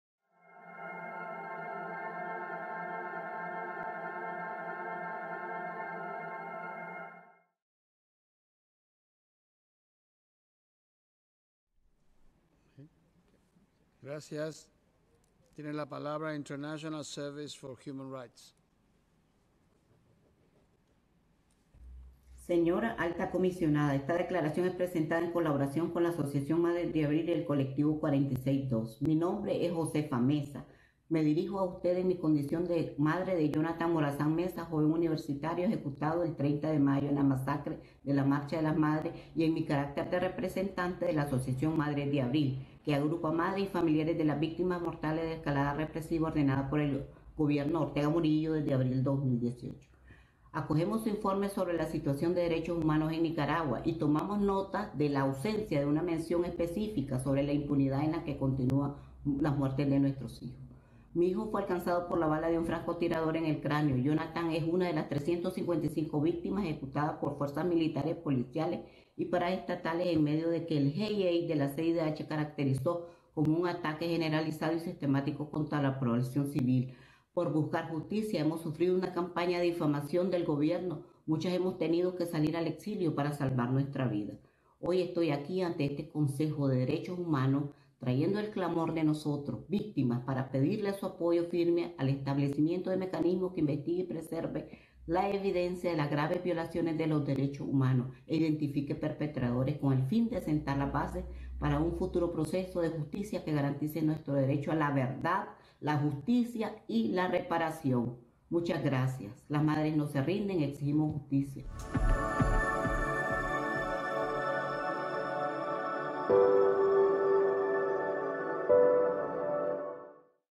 ISHR, Raza e Igualdad y la AMA llaman a la creación de un mecanismo para Nicaragua
Josefa Meza, de la Asociación Madres de Abril (AMA), madre de Jonathan Morazán Meza, joven universitario asesinado durante las protestas masivas de abril de 2018 en Nicaragua, se dirige al Consejo de Derechos Humanos para llamar al establecimiento de un mecanismo de investigación y rendición de cuentas sobre Nicaragua mandatado por la ONU.

La declaración conjunta, en nombre de ISHR, el Instituto Raza e Igualdad, y AMA, fue dada durante el diálogo interactivo con la Alta Comisionada para los Derechos Humanos sobre su informe escrito sobre la situación de derechos humanos en Nicaragua.